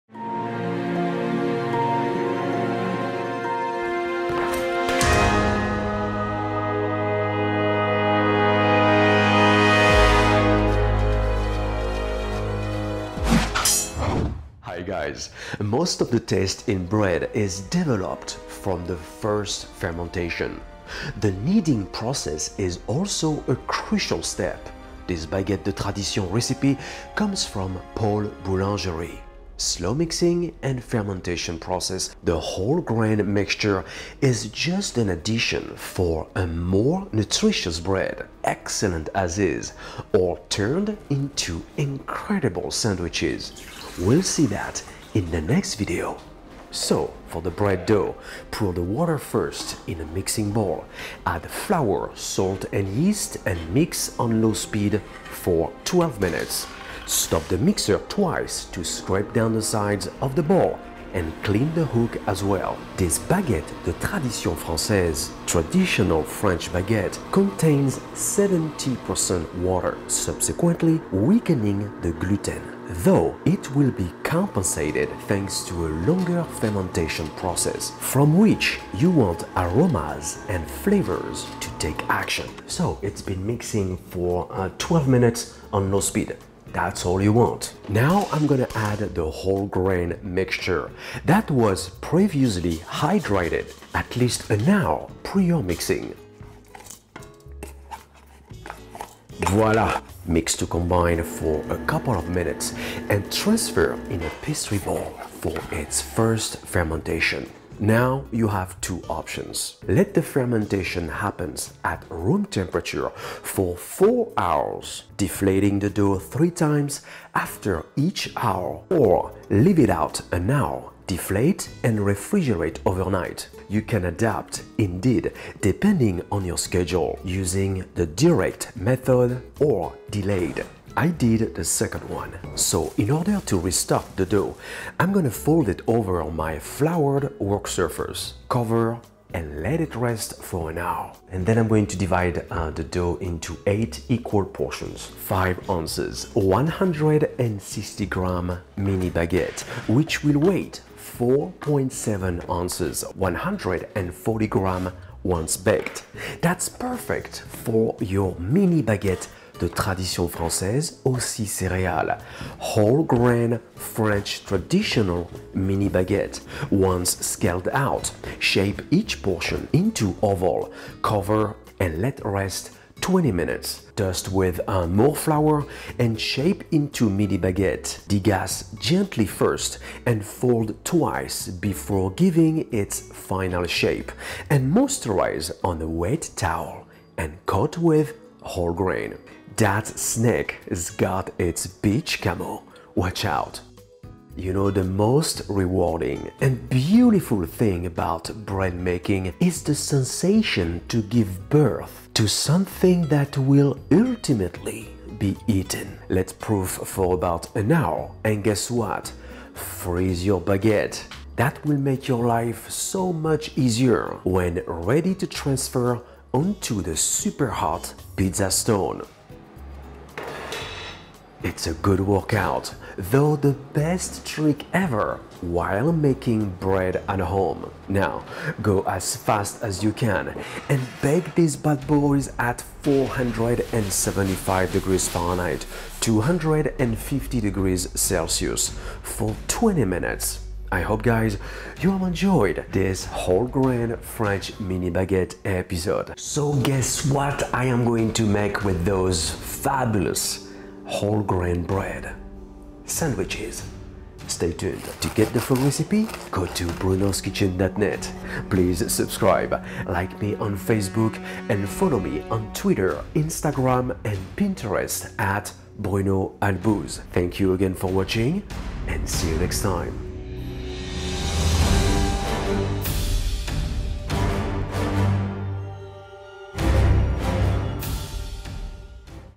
Whole-Grain French Mini Baguette – Bruno Albouze
Excellent and nutritious, here is the whole-grain traditional French mini baguette recipe you were waiting for. The ultimate choice for your future sandwiches.